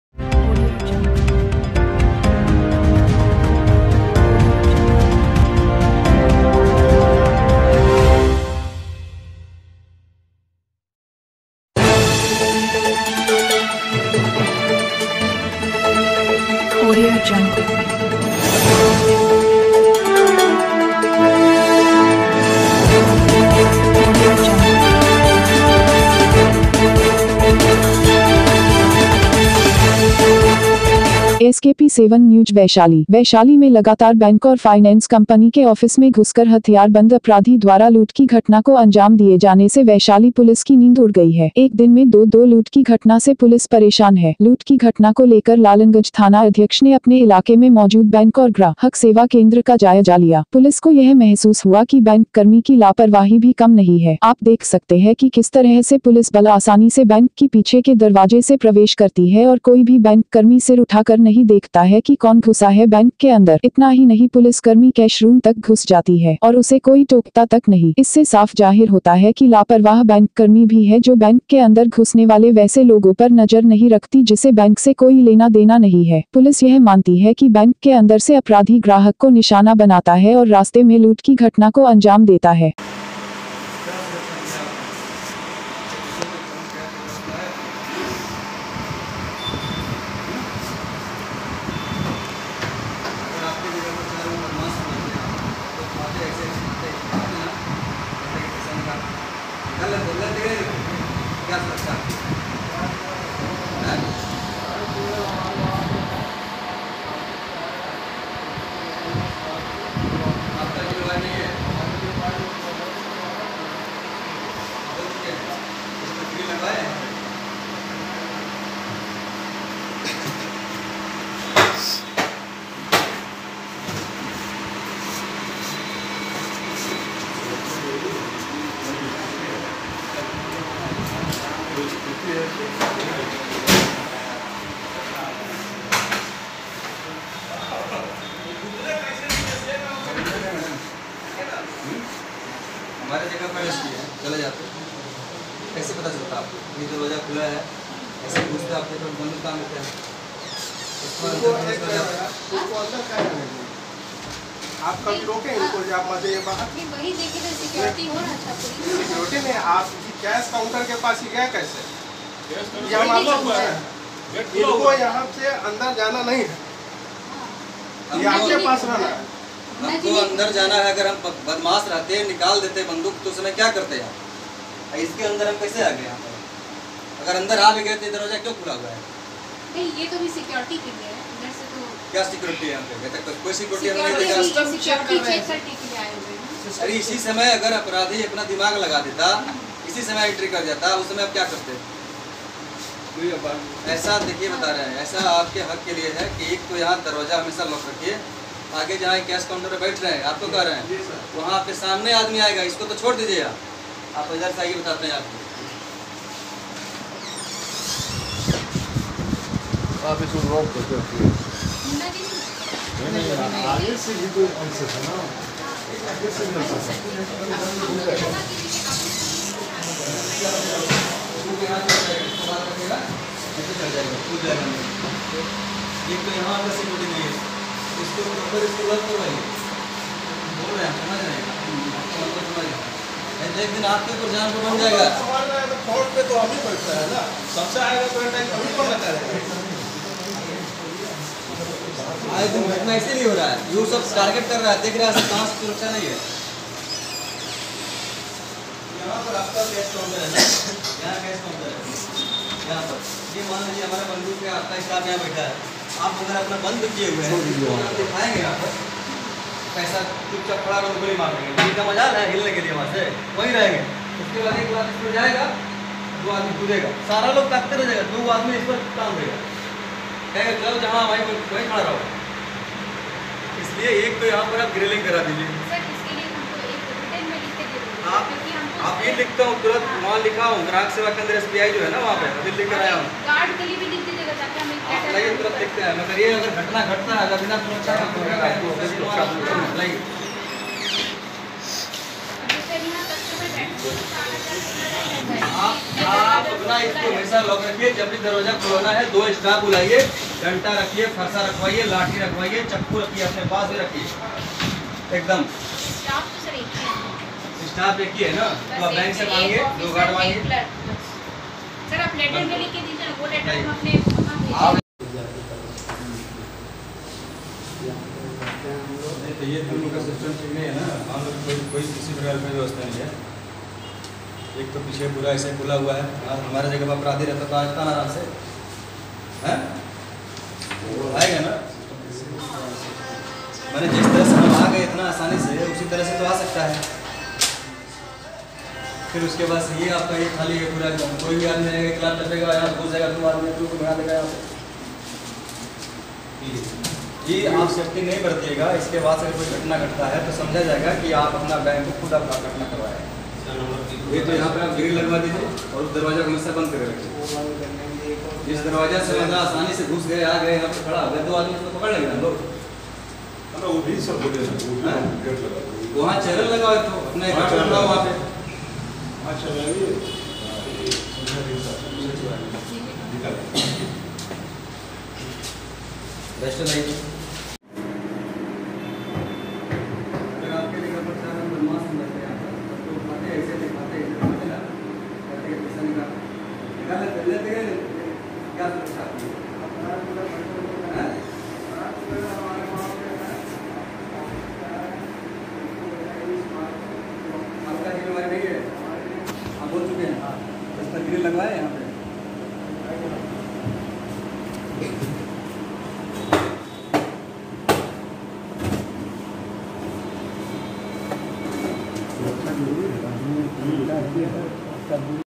बैंक की लापरवाही से बैंक और ग्राहक की होती है लूट, पुलिस पहुंच गई बैंक के पिछले दरवाजे से कैश तक
अधिकारी को इसकी भनक तक नहीं देखिए लाइव वीडियो